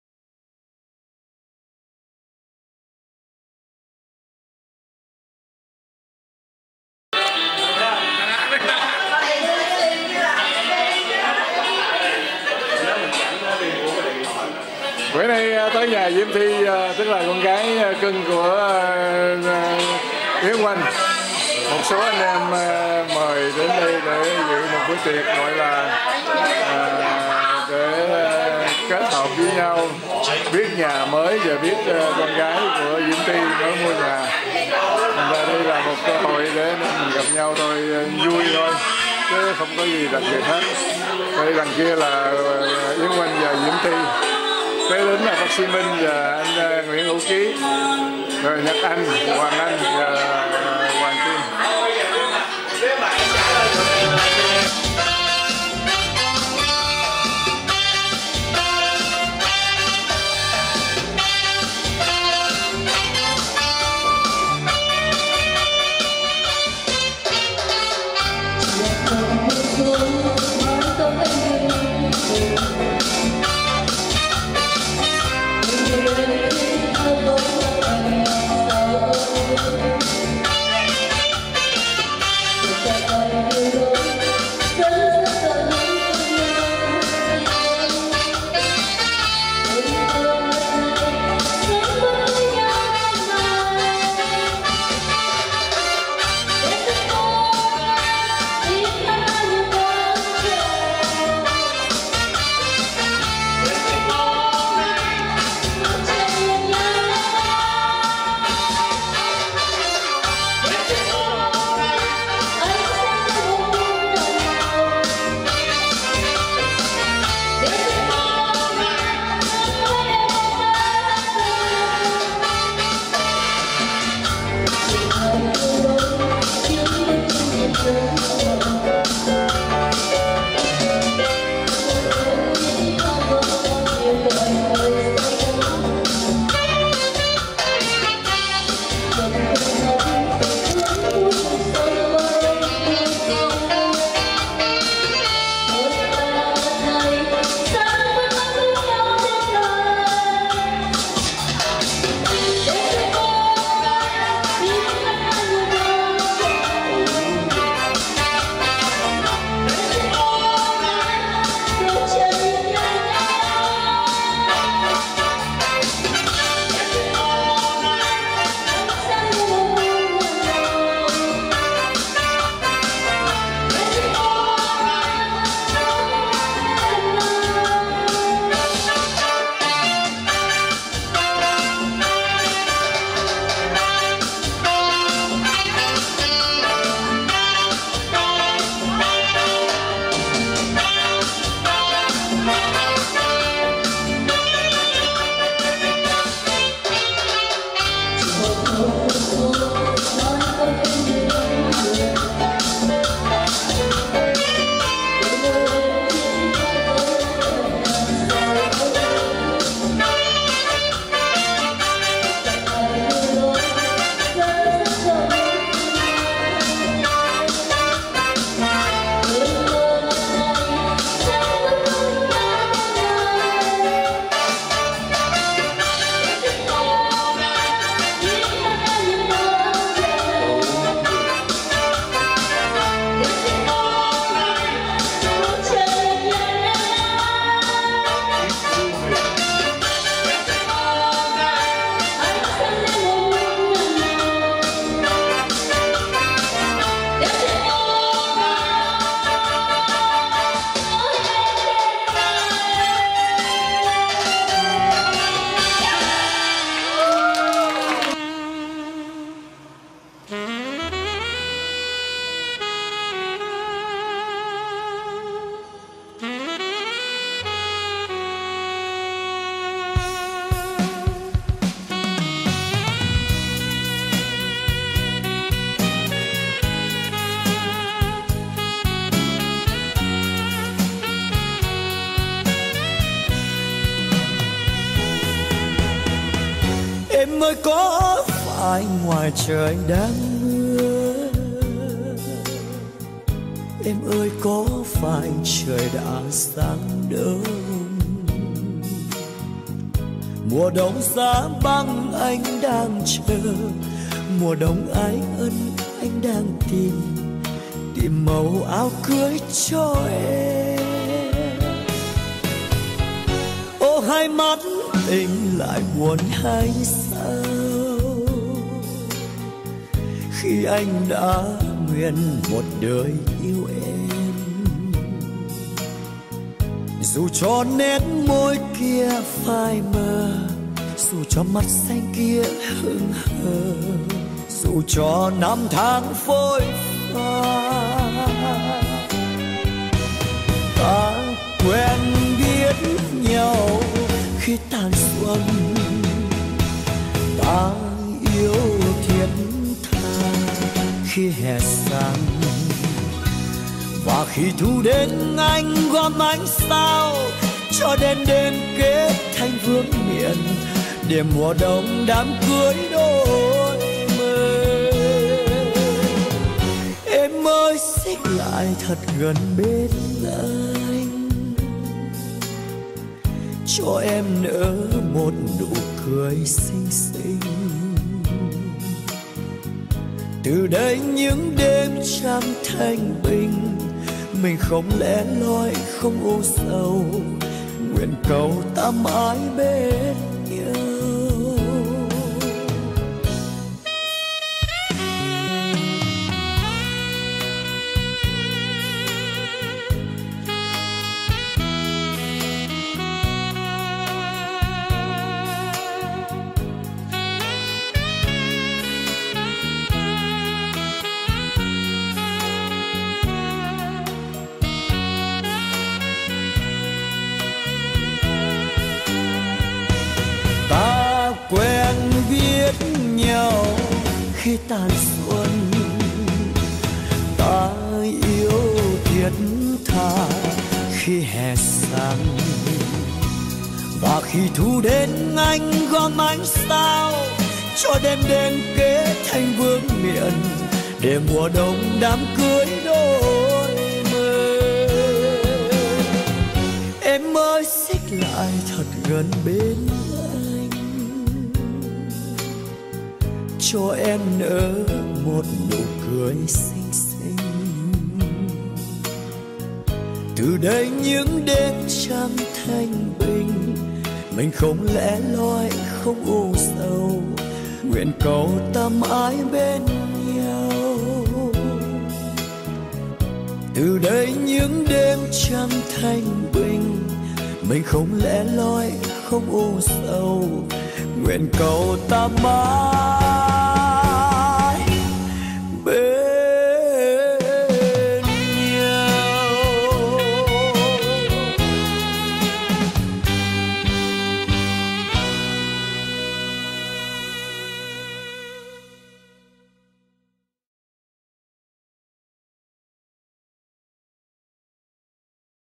House Warming!!
Video clip và slide show hình ảnh gia đình Yến-Oanh và thân hữu họp mặt ăn tối mừng nhà mới của Matthew Barnes & Diễm Thy.